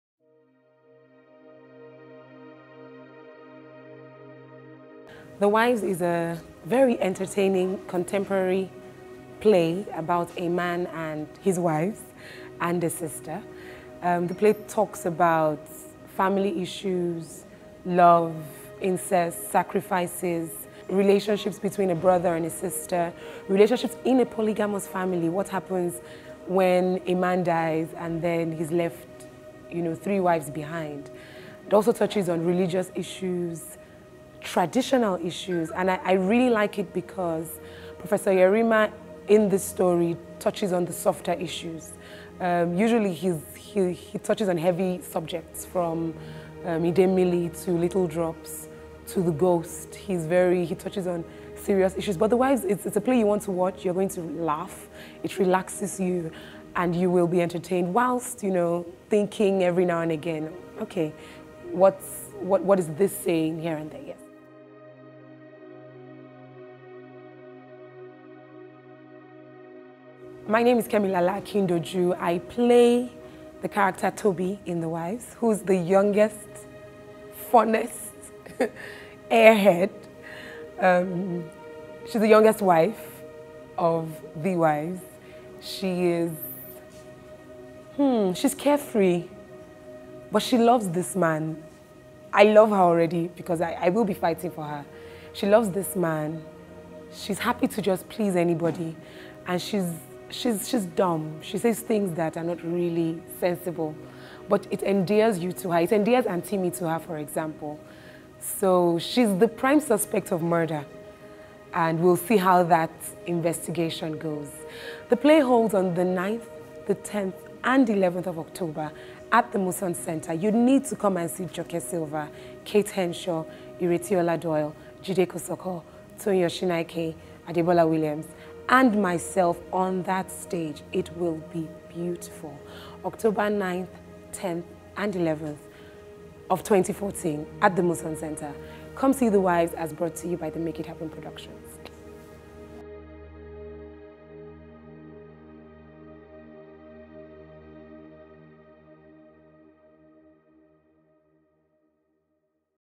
Kemi Lala Akindoju speaks about The Wives
On the 9th, 10th and 11th Lala Akindoju ,Joke Silva, Kate Henshaw Iretiola Doyle, Jide Kosoko will be bringing the stage alive at the Muson centre in AHMED YERIMA'S ' THE WIVES'
Directed by Seke Somolu
Produced by Kemi Lala Akindoju